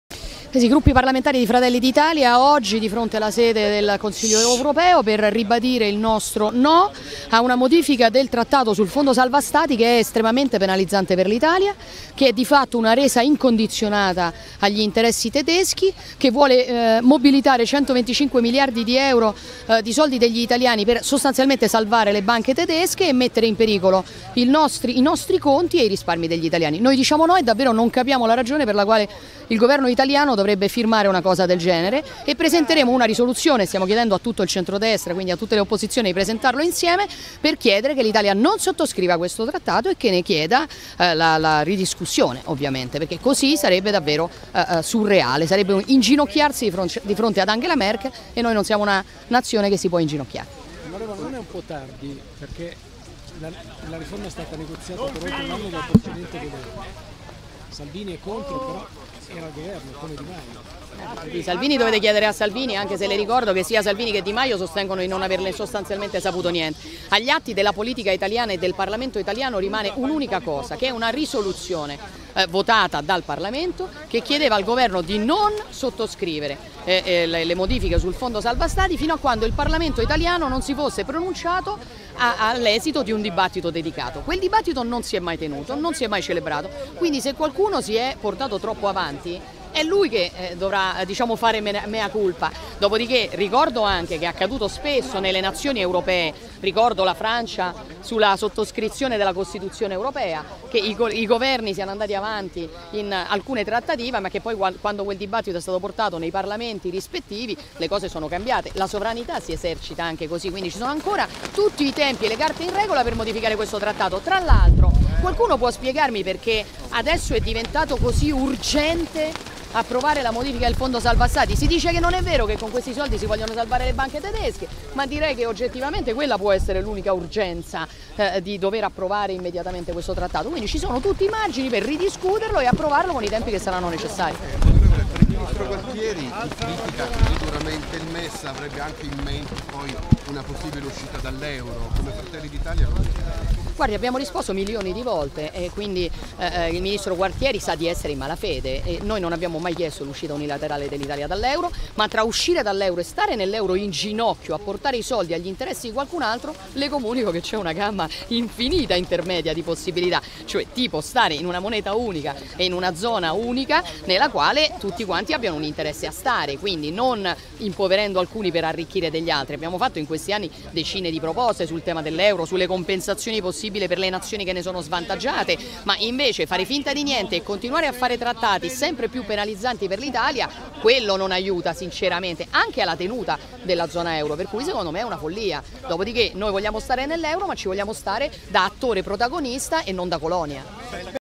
Mes, Meloni: "Riforma è inginocchiarsi a Merkel"
"Gualtieri in malafede, mai chiesta uscita unilaterale euro"

POLITICA (Bruxelles). "I gruppi parlamentari di Fratelli d'Italia, oggi, di fronte alla sede del Consiglio europeo per ribadire il nostro No a una modifica del Trattato sul fondo salva Stati che è estremamente penalizzante per l'Italia, che di fatto è una resa incondizionata agli interessi tedeschi, che vuole mobilitare 125 miliardi di euro di soldi degli italiani sostanzialmente per salvare le banche tedesche e mettere in pericolo i nostri conti e i risparmi degli italiani. Noi diciamo No e davvero non capiamo la ragione per la quale il governo italiano dovrebbe firmare una cosa del genere e presenteremo una risoluzione. Stiamo chiedendo a tutte le opposizioni di presentarlo insieme per chiedere l'Italia non sottoscriva questo trattato e che ne chieda la ridiscussione perché così sarebbe davvero surreale, sarebbe inginocchiarsi di fronte ad Angela Merkel e noi non siamo una nazione che si può inginocchiare". Così la presidente di Fratelli d'Italia, Giorgia Meloni, nel sit-in contro la riforma del Mes. (Giulia Torbidoni/alanews)